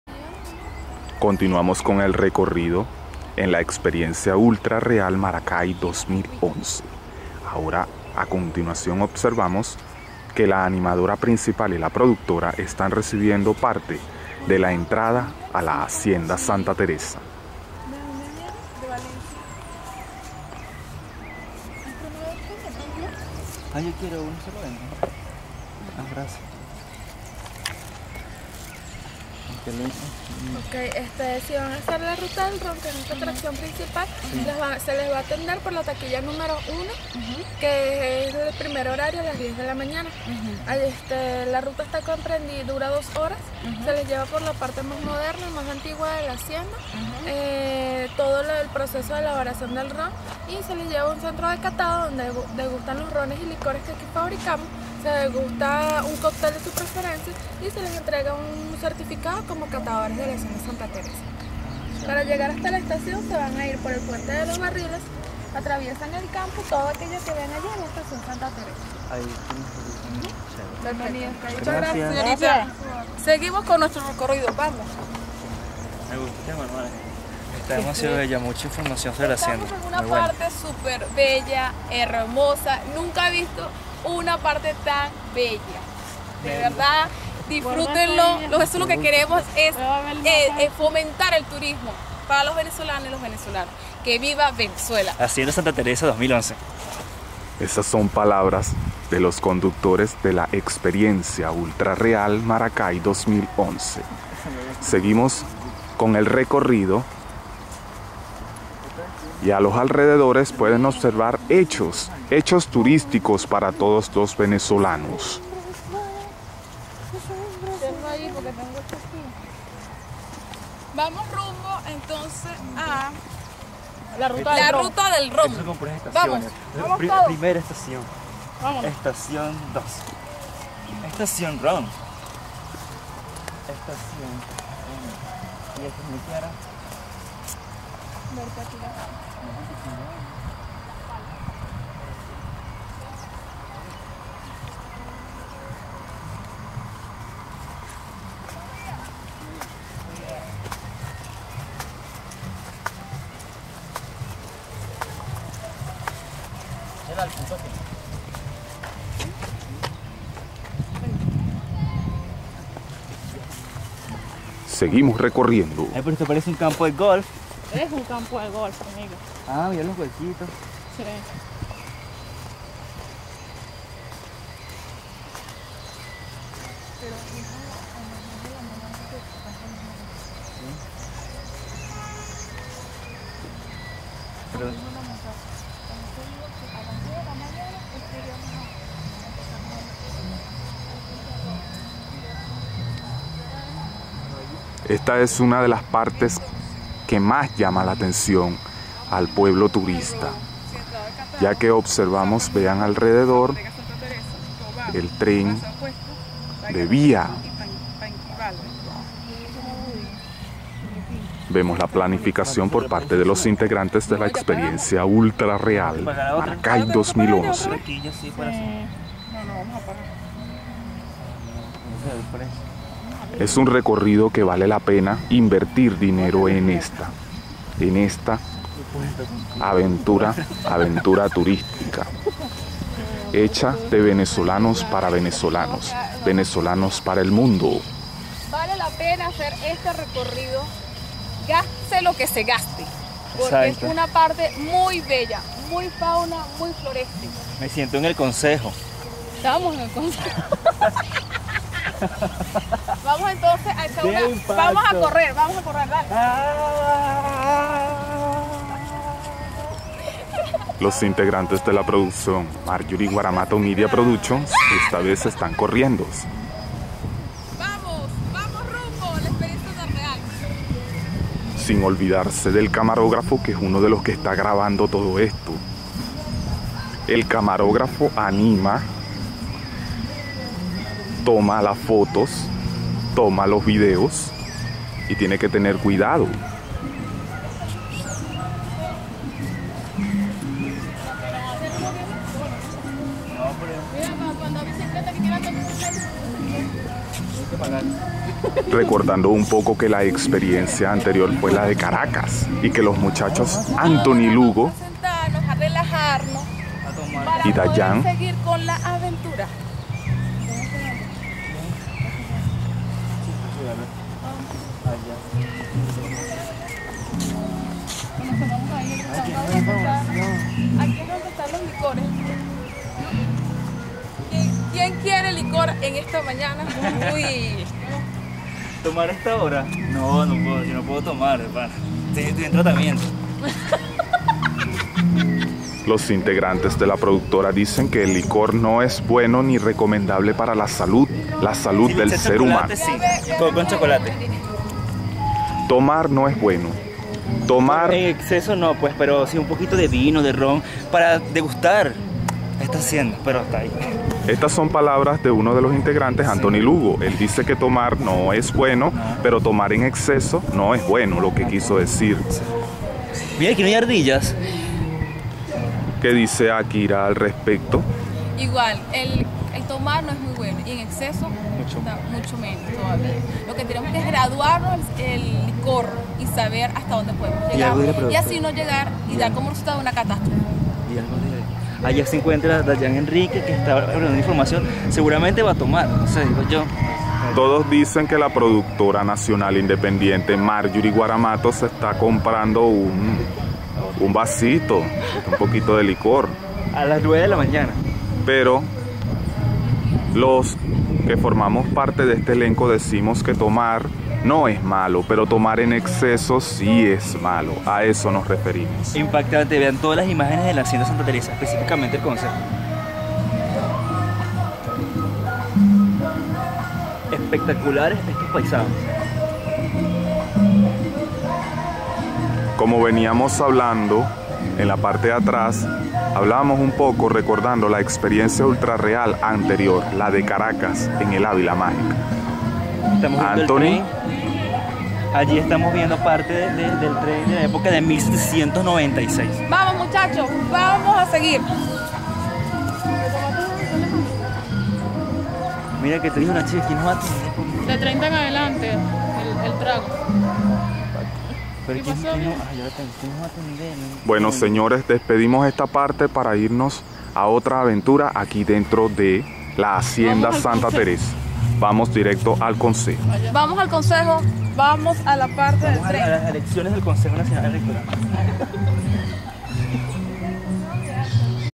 EXPERIENCIA ULTRA REAL MARACAY 2011 (RECORRIDO COMPLETO DENTRO DE LA HACIENDA)
DESDE EL CONSEJO LA PRODUCCIÓN ENTRO Y AQUI SE OBSERVAN LUGARES NATURALES.